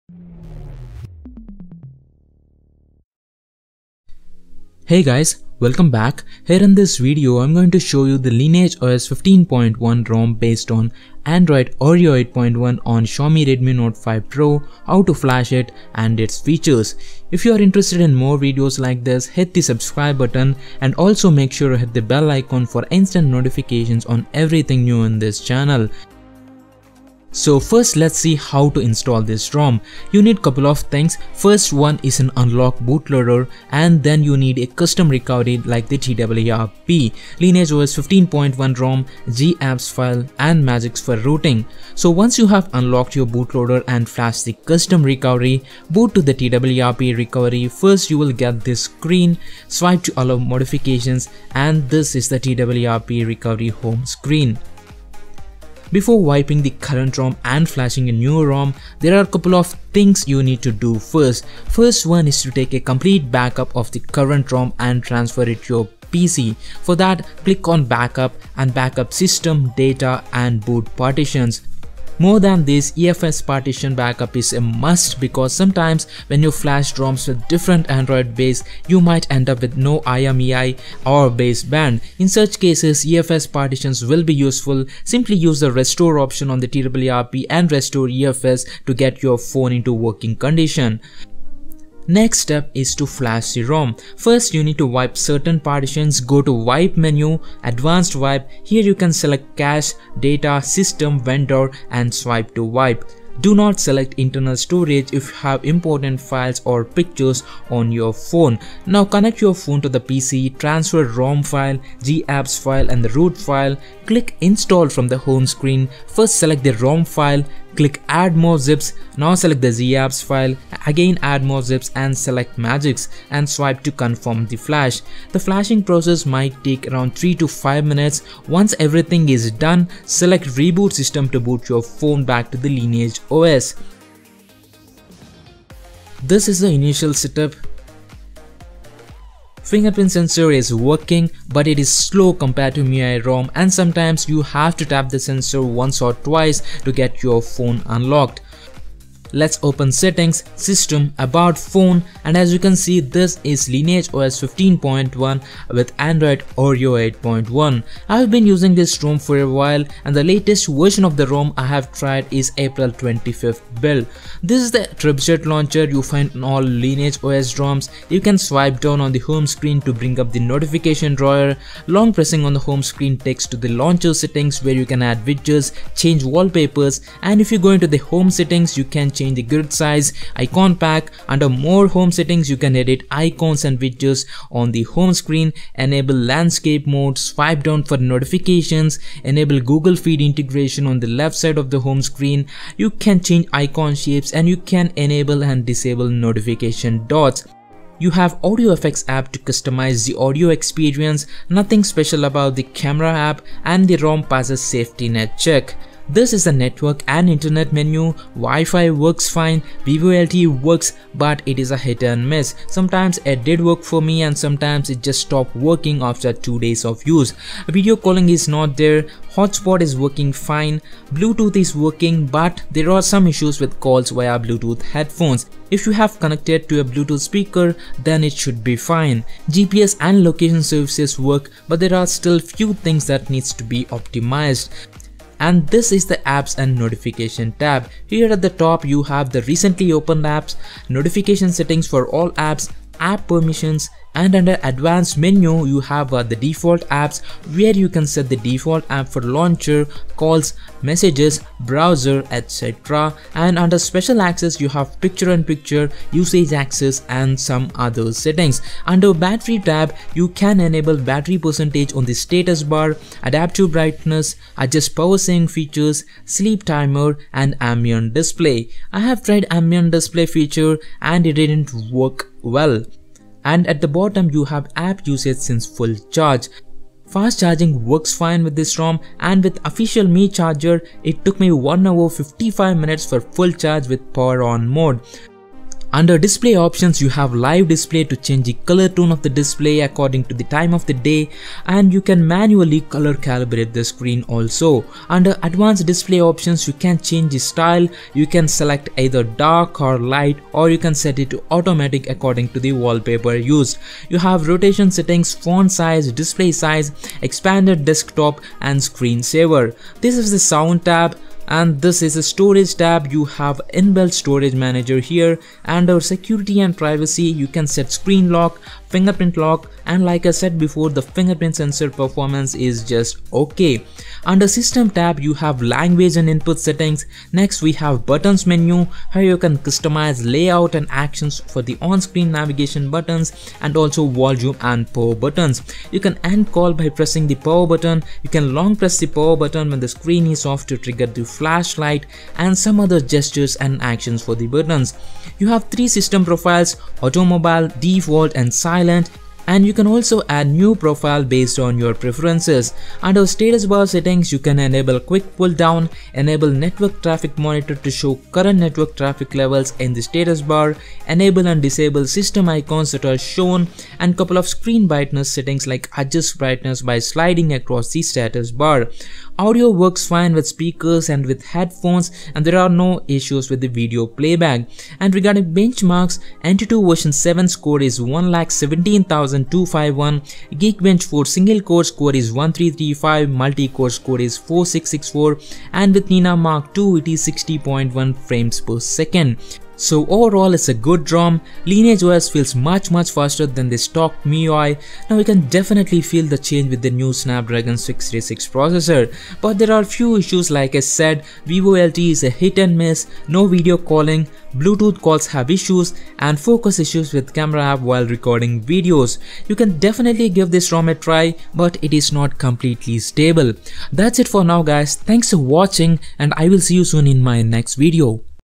Lineage OS 15.1 [Oreo 8.1] on Xiaomi Redmi Note 5 Pro | Review
Lineage OS 15.1 ROM based on Android Oreo 8.1 for Xiaomi Redmi Note 5 Pro | How to Install + Features + Review

Root (Magisk) : chrome-extension://ngpampappnmepgilojfohadhhmbhlaek/captured.html?back=1